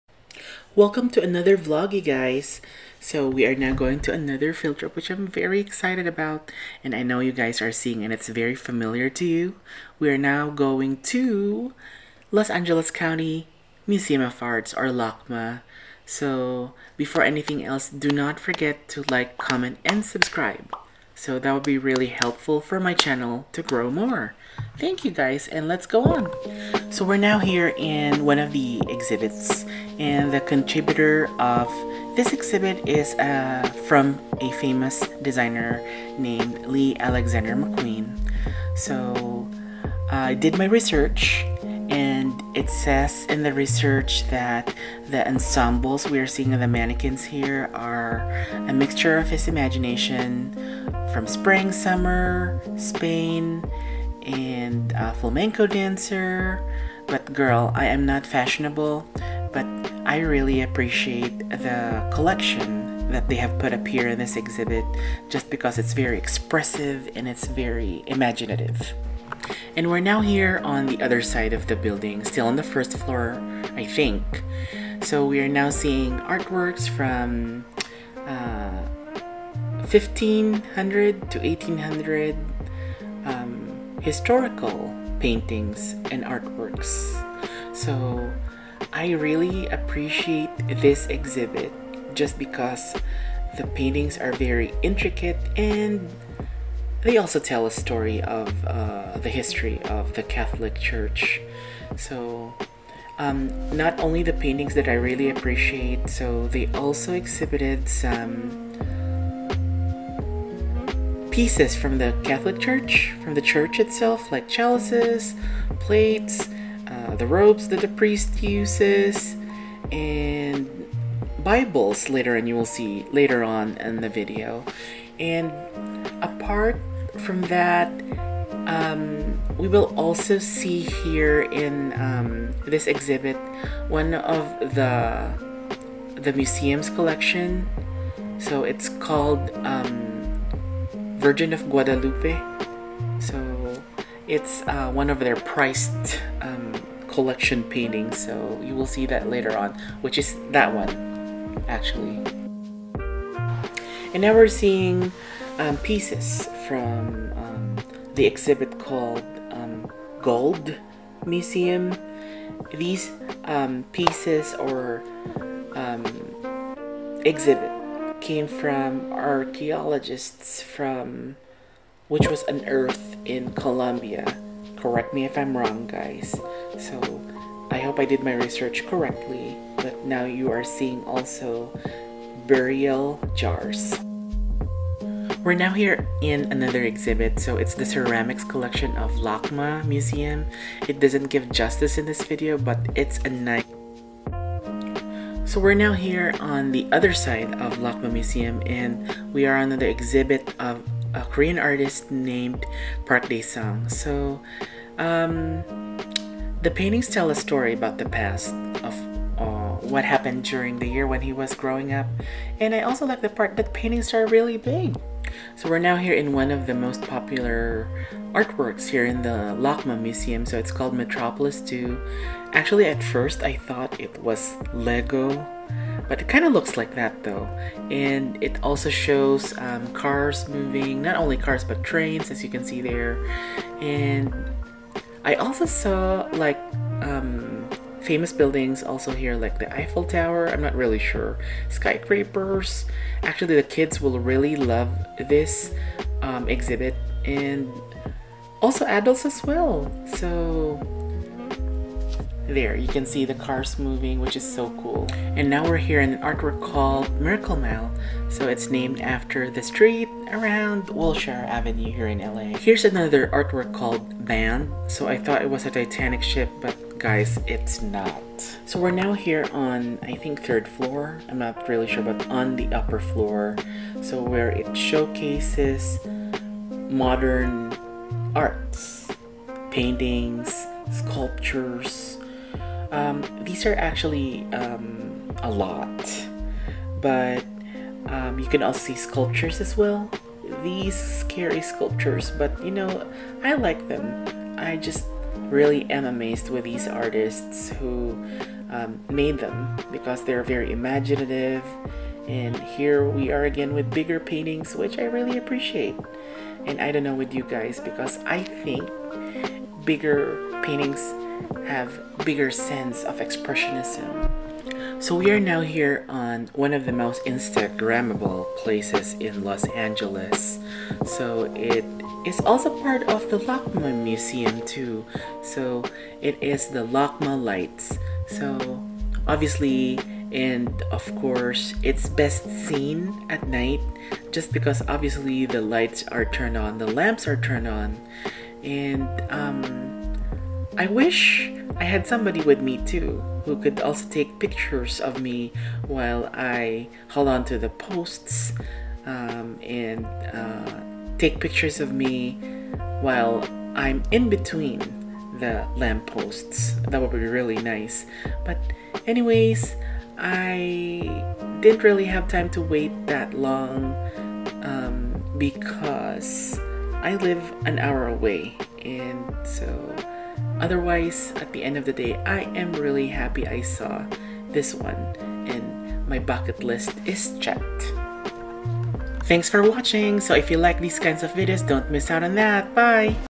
Los Angeles County Museum of Art LACMA tour * see Urban Lights, Lee Alexander McQueen, & more *
Mabuhay friends! In this vlog, I am going to take you with me as I go to one of the popular tourist spots in Los Angeles, LACMA or Los Angeles County Museum of Arts. You’ll see contemporary and modern art works, historical church paintings, the famous light posts, and many more. :)

Time Stamps:

00:26 - Lee Alexander Mcqueen exhibit
01:09 - Catholic Church 1500 to 1800 exhibit
02:31 - Virgin of Guadalupe art painting
02:34 - Gold Museum artifacts exhibit
03:05 - Ceramics Collection exhibit
03:14 - Park Dae Sung paintings exhibit *large paintings*
03:35 - Metropolis II exhibit *super fave*
04:15 - Miracle Mile lights exhibit
04:22 - Band exhibit
04:28 - modern art works
05:14 - Urban Lights *instagrammable and also my fave*